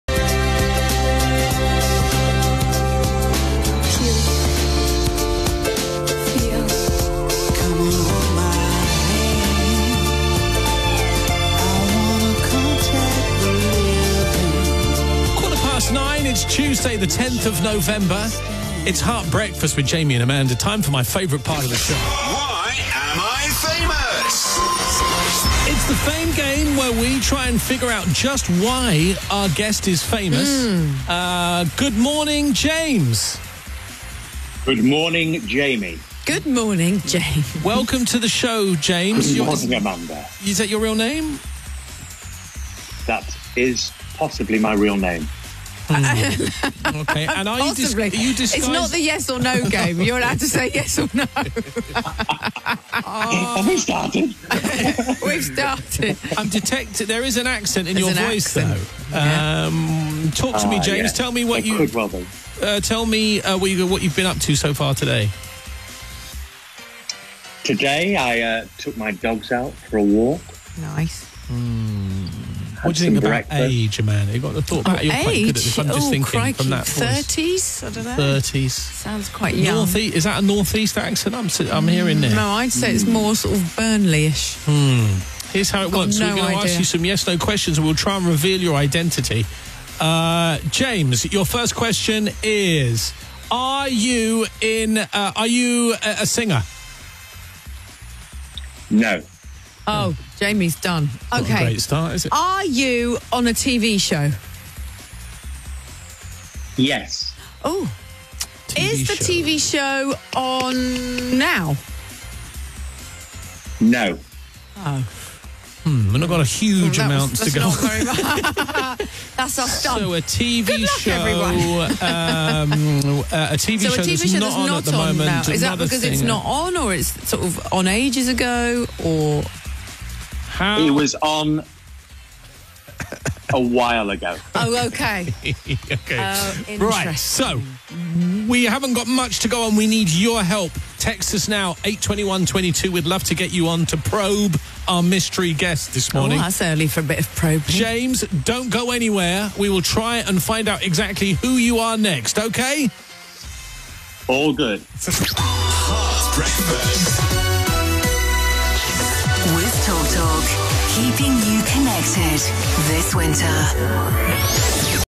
ThisIsHeart best moments or any moments Jamie and Amanda play why am I famous game this is part 1
This is the next ThisIsHeart best moments or any moments it’s the next day of ThisIsHeart best moments or any moments Jamie and Amanda play the game called why am I famous any guesses on who it might be and this is part 1 subscribe to my YouTube channel my main YouTube channel is in the link below the subscription enjoy